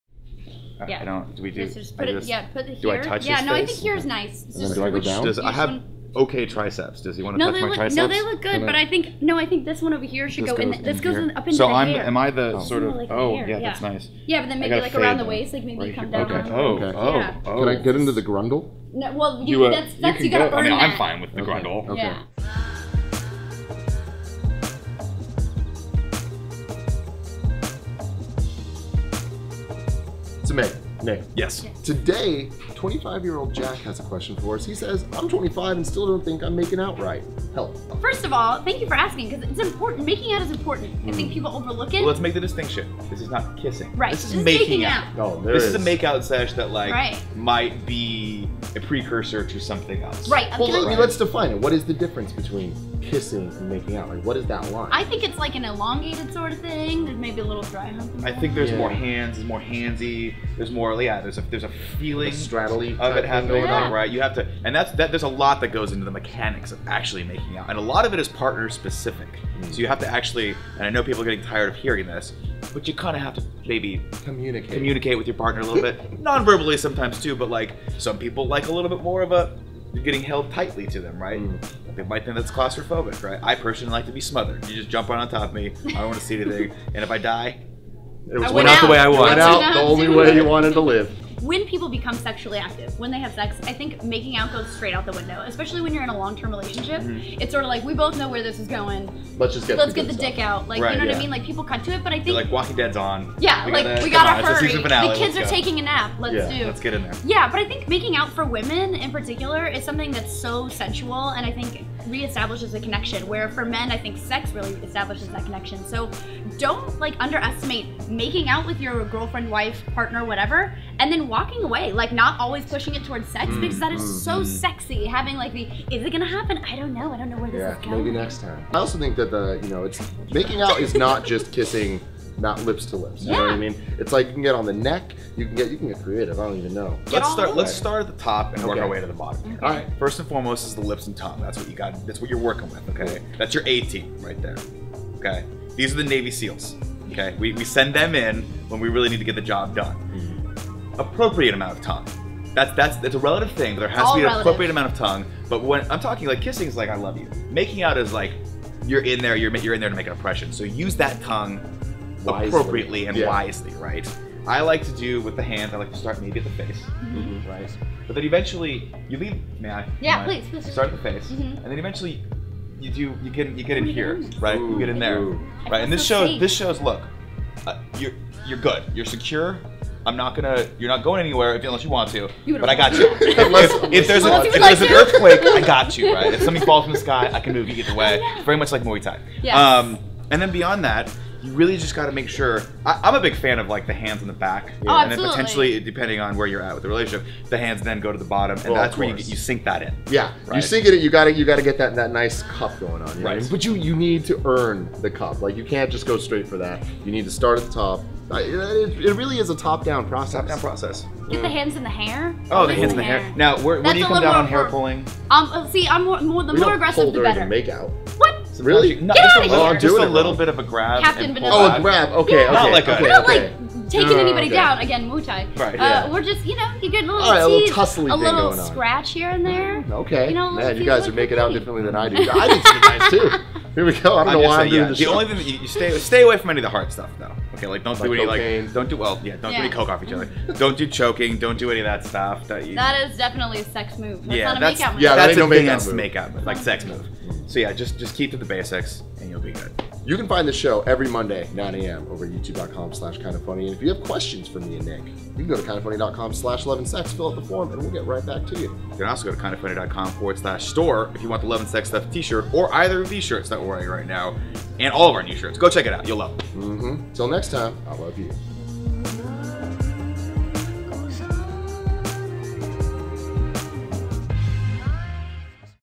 How To Makeout (w/Meg Turney) - Love & Sex Stuff
You think you know how, but you have no idea.

Each and every Monday at 9AM PST Tim Gettys and Nick Scarpino answer your questions about dating and relationship advice. cause that sounds like a good idea.